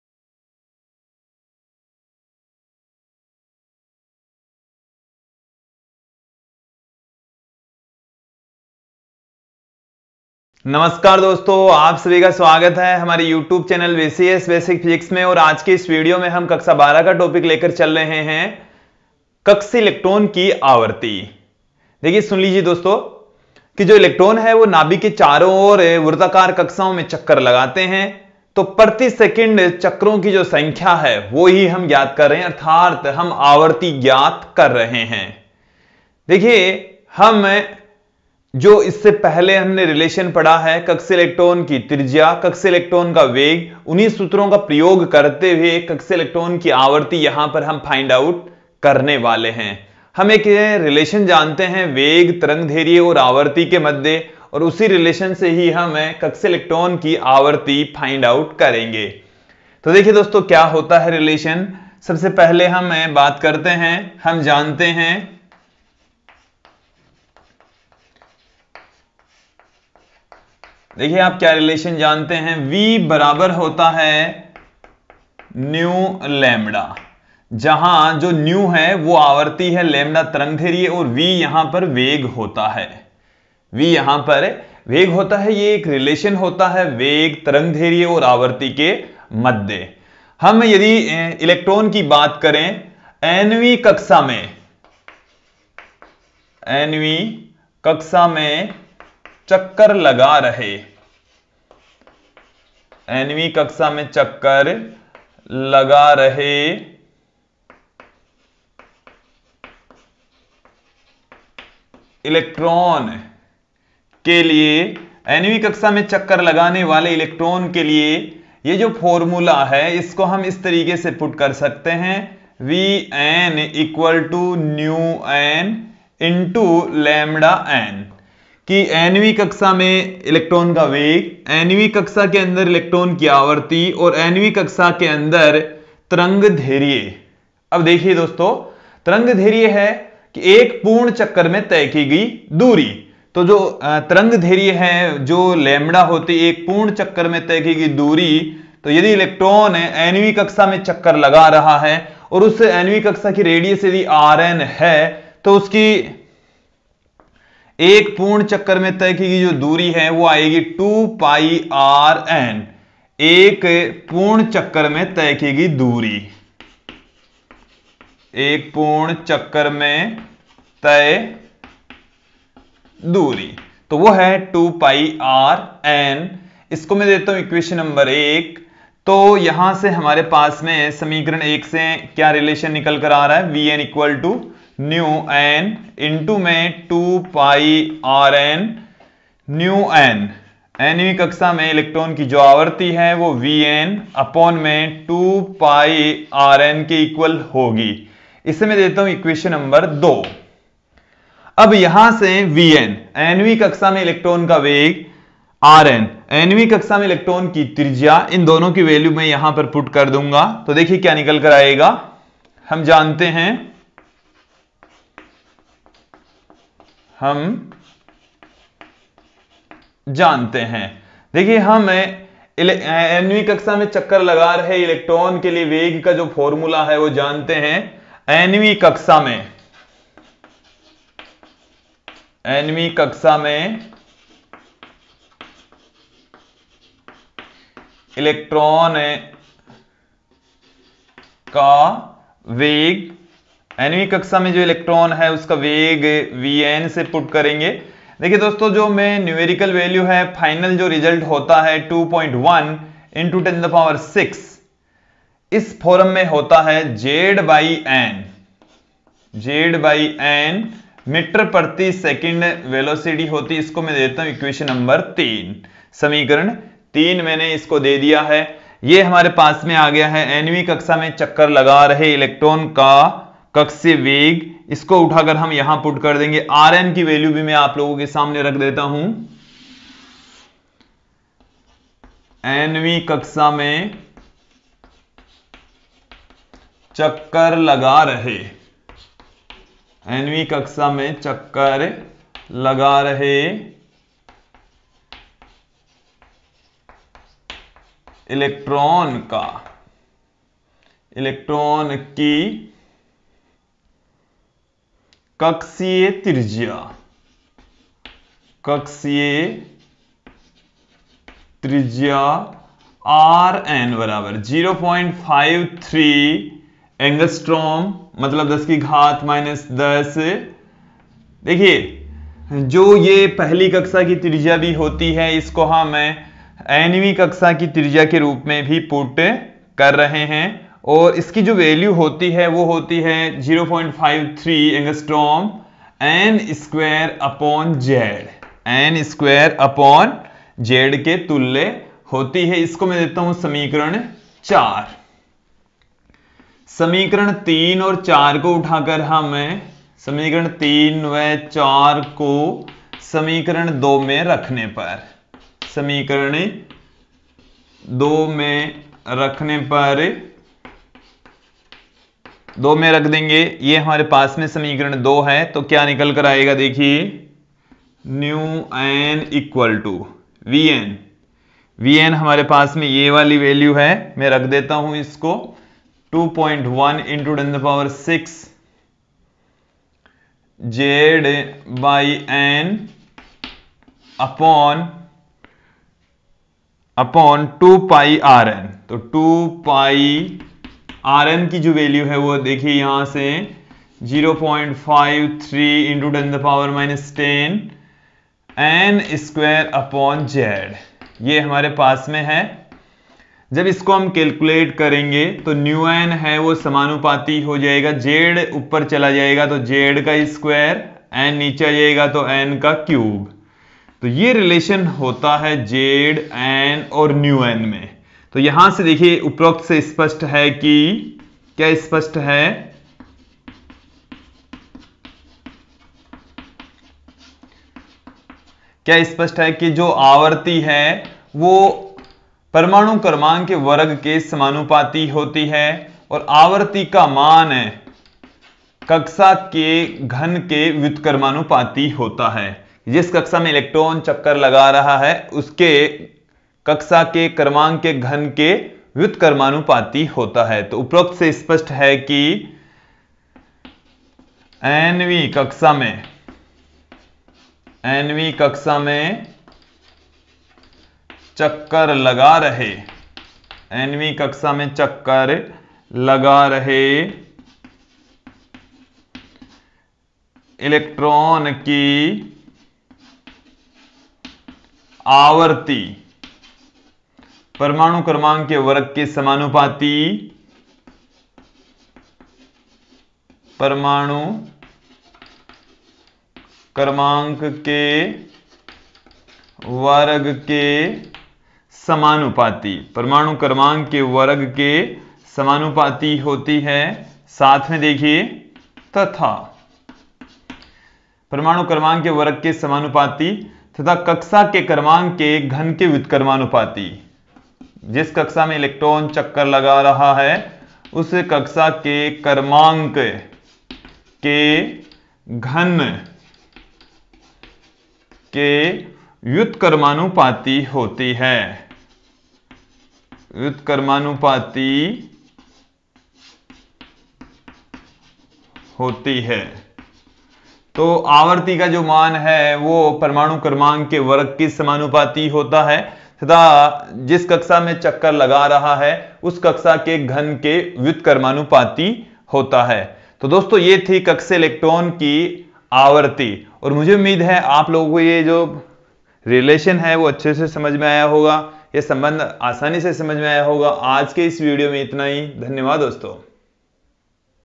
12th-224 कक्षीय इलेक्ट्रान की आवृत्ति |frequency of orbital electron | application of bohr's model
Welcome to the our youtube channel
BCS BASIC PHYSICS
Chapter - atomic physics
Topic - frequency of orbital electron

In this video we study about frequency of orbital electron. This video is useful for class 12th physics students and navy airforce students and neet jee students.